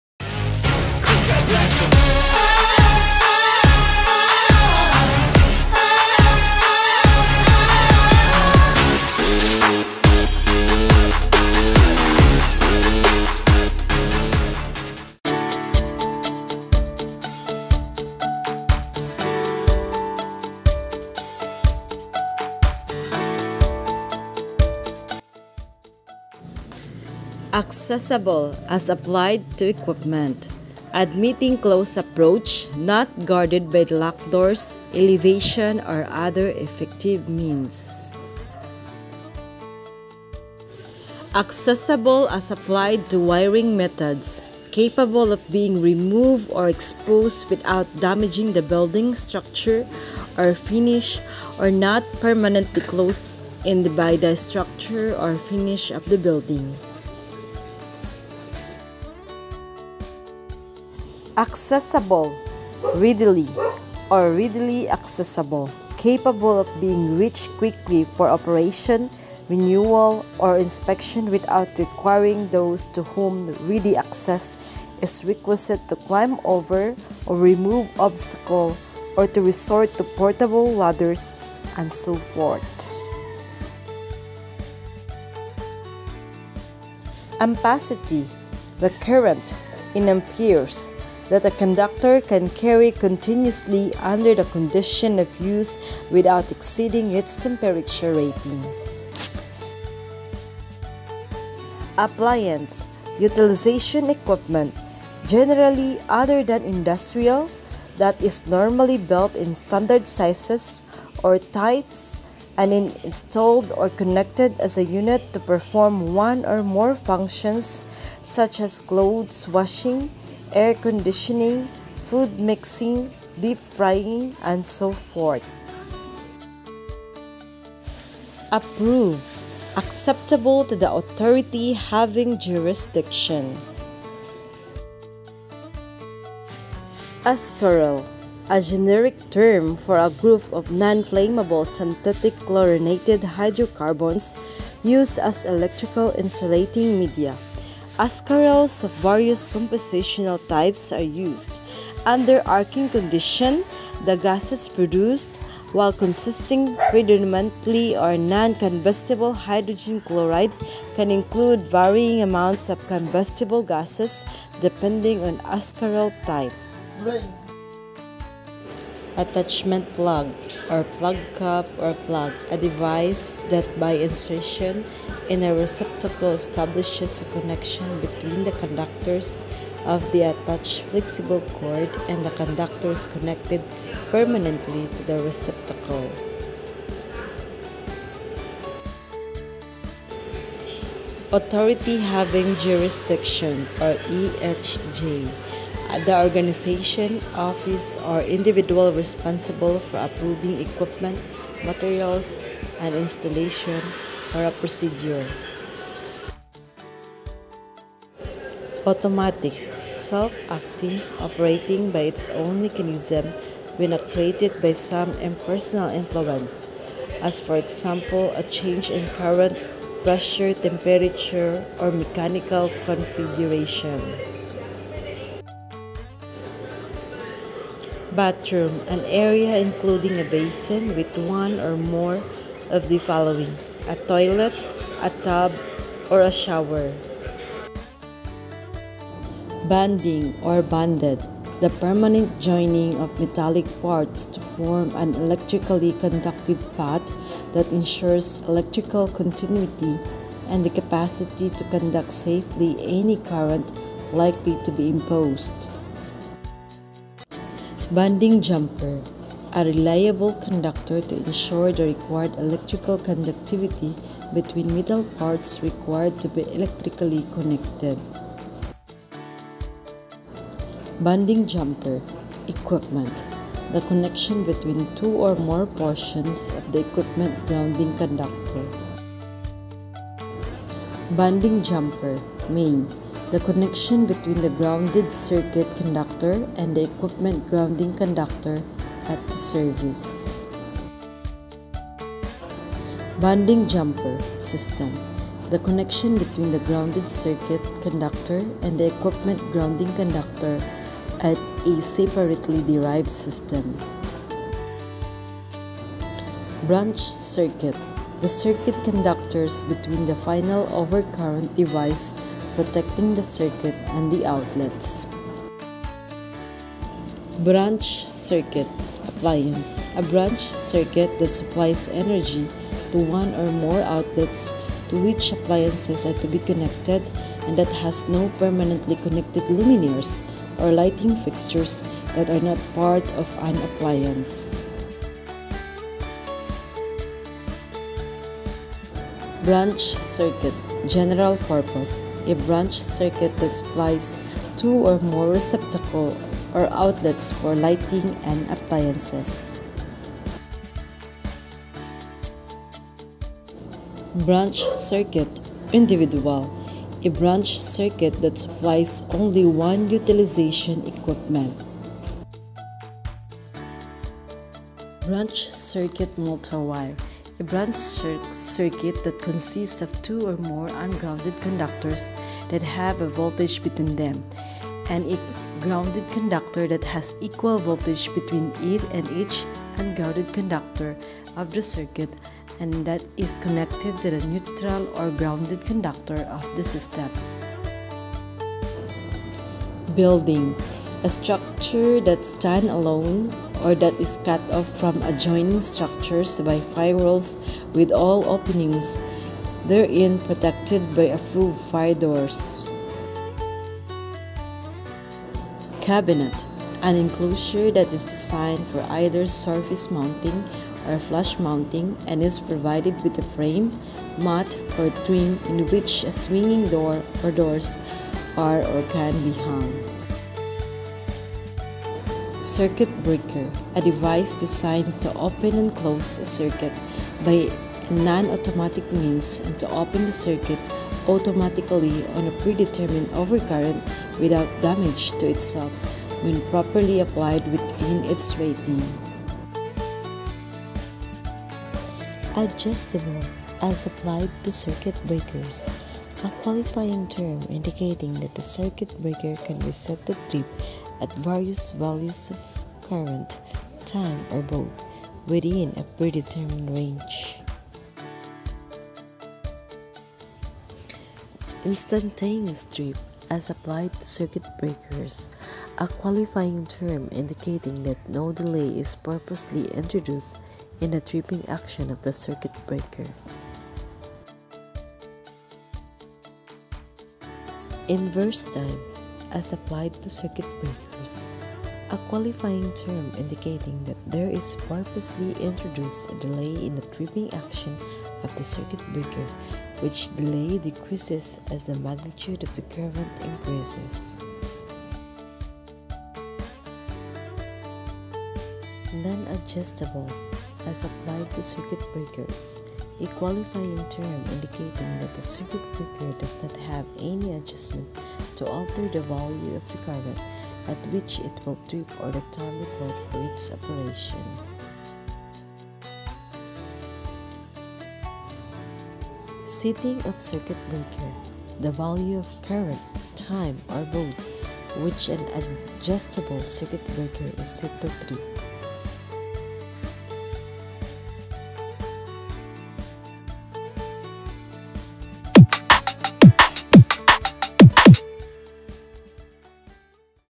PEC Part 1 [ Definition of Terms ] Philippine Electrical Code
PEC Definition of Terms| Philippine Electrical Code

For Registered Electrical Engineer and Registered Master Electrician Board Examination